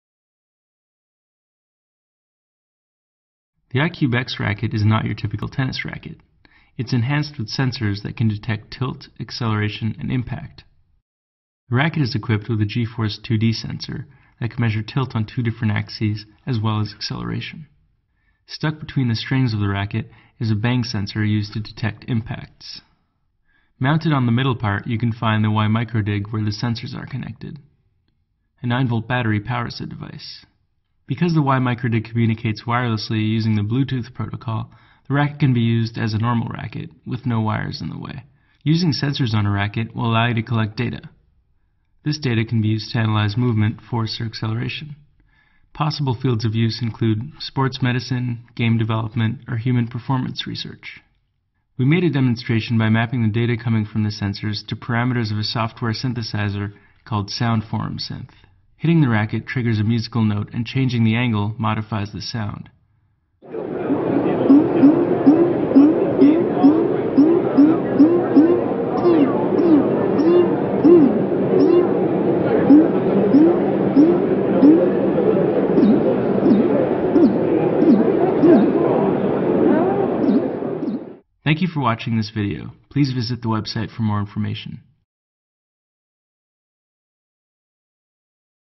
Racquet motion sensing
Discover what happens when you hit a ball with your tennis racquet by instrumenting it with I-CubeX sensor technology !